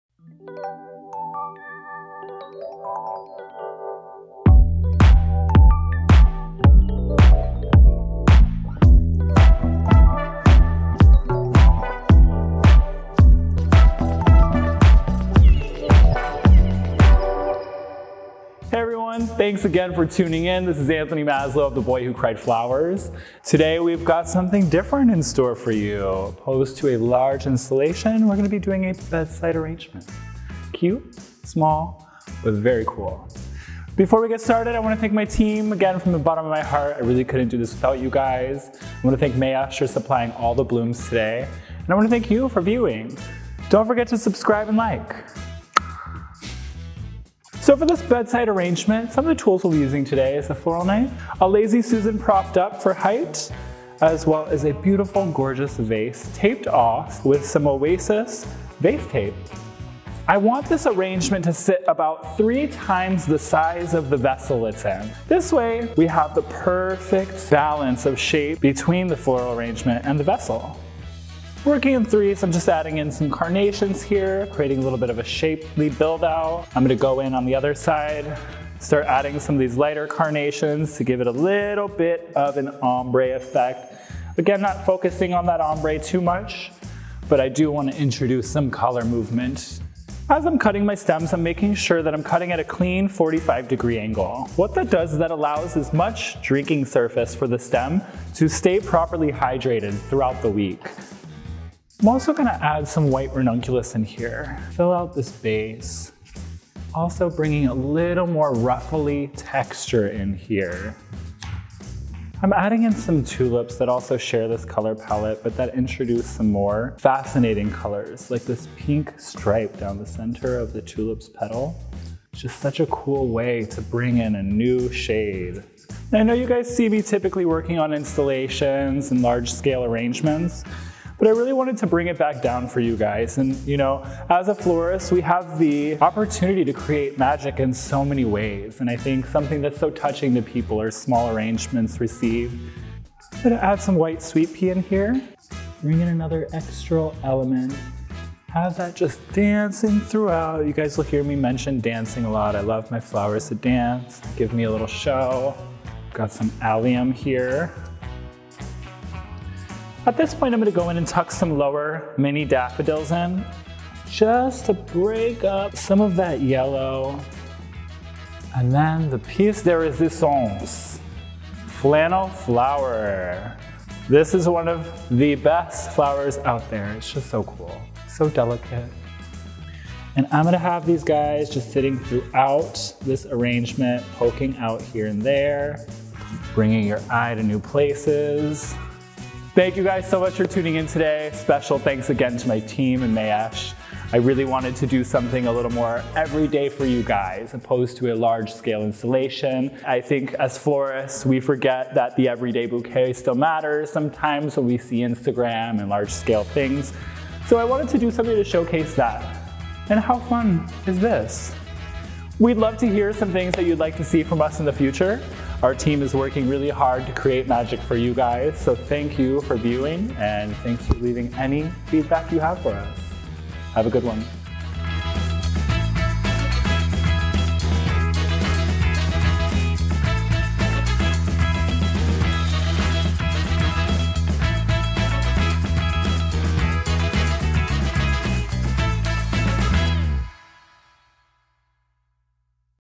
Mayesh Design Star: Bedside Floral Arrangement
For Anthony's next Mayesh Design Star video, he changes things up by creating a cute, small but very cool floral bedside arrangement. Starting by taping his small vase, Anthony begins creating a shape using carnations. Anthony continues on to explain why it is important to cut the stems in a 45-degree angle. Afterward, he adds tulips, gerberas, daffodils, allium, and flannel flower creating a sweet everyday bouquet perfect for your clients' bedside tables or other small spaces.

Share with us your thoughts on Anthony's newest video in the comments!

FEATURED PRODUCTS*

Allium Snakeball
Ranunculus White
Carnation Polimnia
Carnation Cream
Flannel flower
Gerbera Pasta Carbonara Cream
Mini White/Yellow Canaliculatus Daffodil

* Please note that due to the fact that this video was shot months ago and we are a bit behind on our production schedule, some of the seasonal flowers may not be available. Also, note that peach poppies can be extremely limited even when they are in season.

FLORAL SUPPLIES

Oasis Waterproof Tape
Lazy Susan
Floral Knife
Shears
Small Vessel